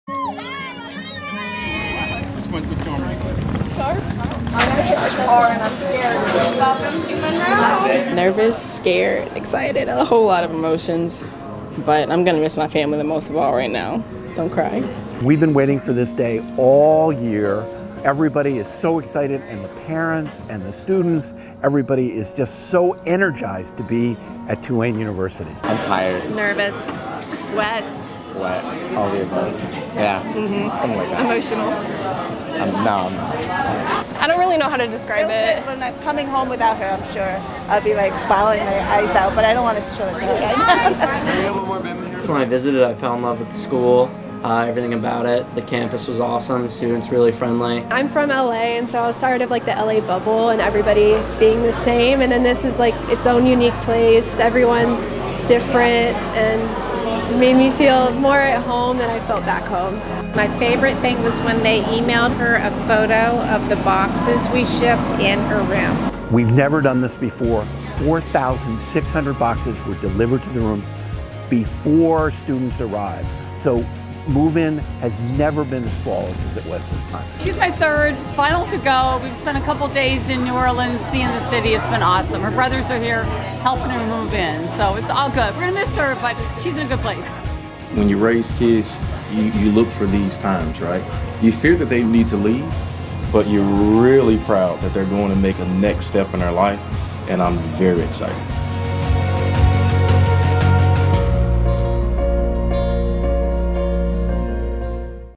Tulane Move-In Day 2019
First-year students moved into their new homes as Tulane University welcomed the Class of 2023.